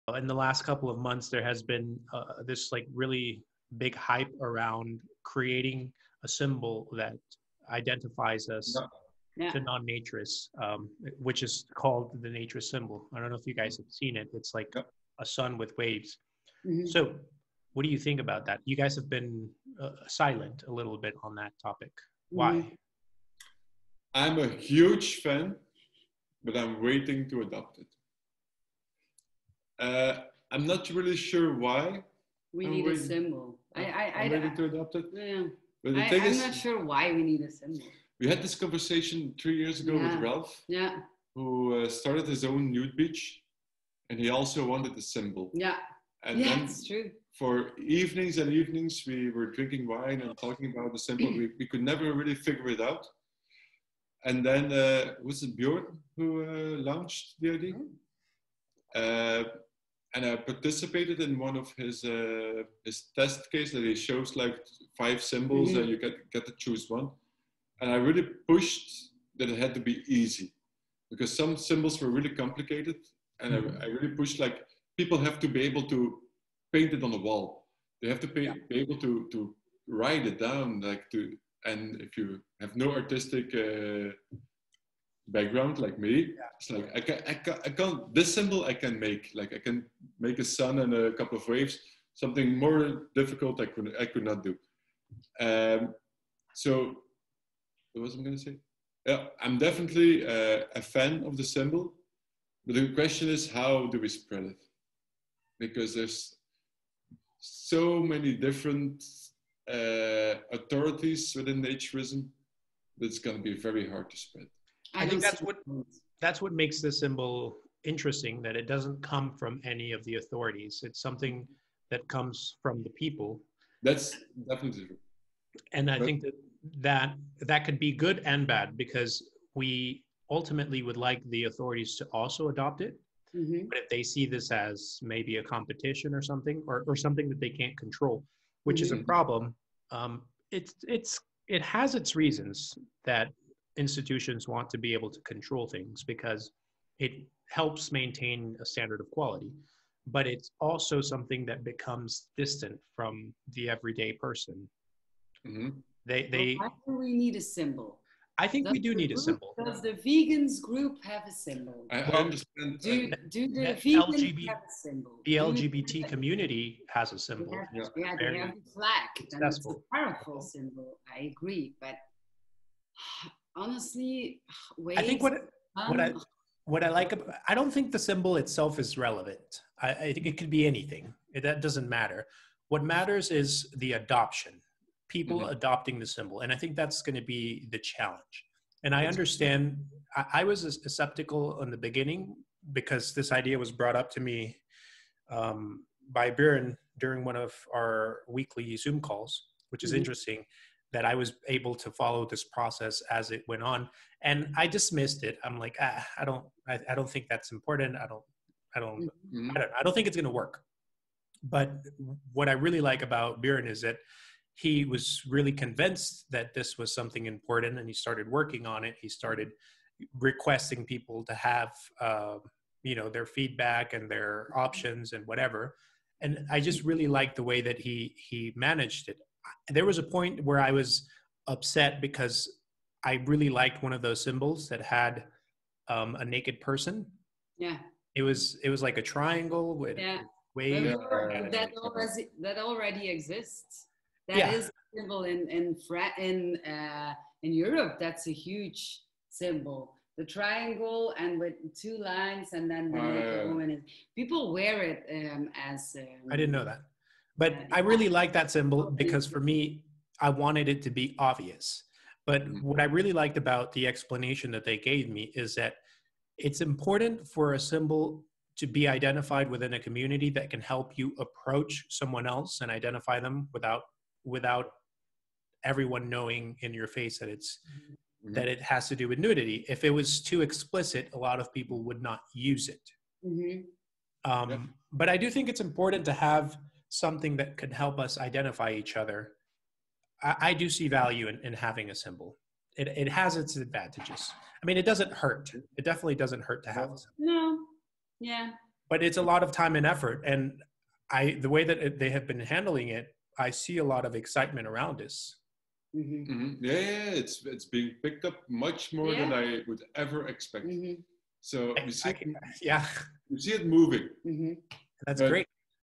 Nick and Lins discuss The Naturist Symbol
During our Naked Convo with: Naked Wanderings we discussed The Naturist Symbol.